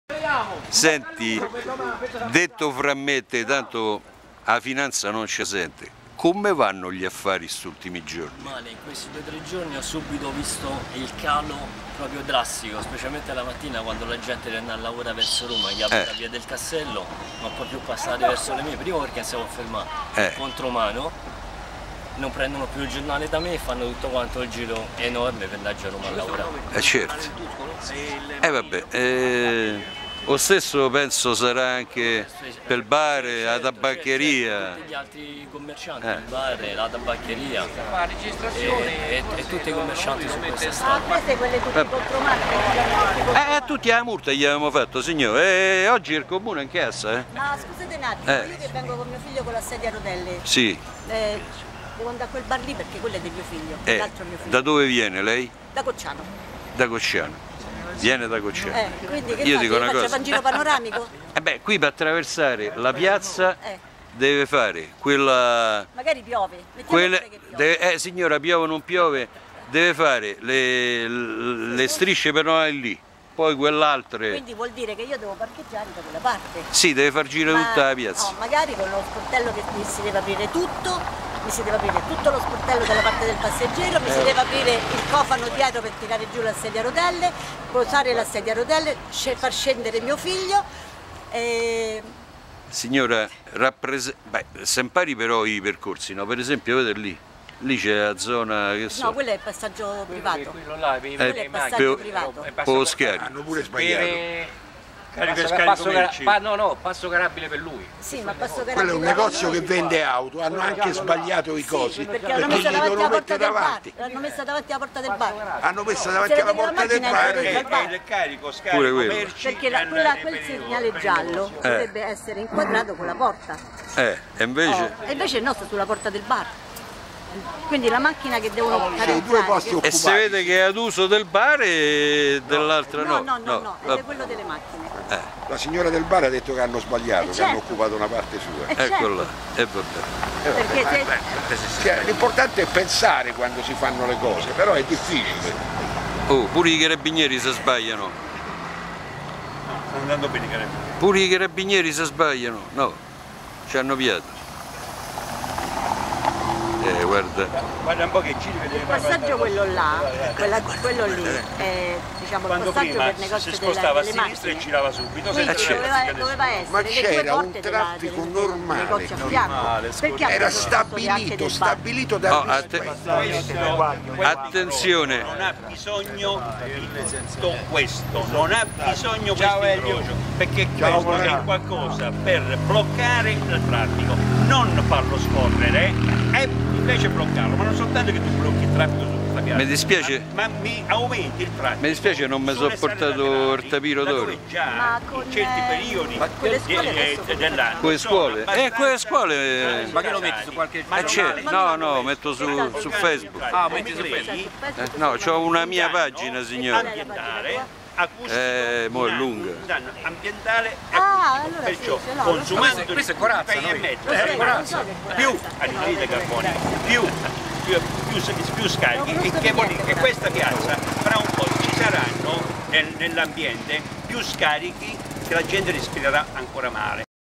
FESTIVAL di LARGO PENTINI 9 - Il GENIUS LOCI che ammazza il commercio
La denuncia di GUIDO.  E dire che sarebbe basato restringere la BASE ATTERRAGGIO PER CHI VOLA ALTO e lasciare quella viabilità a doppio senso di marcia.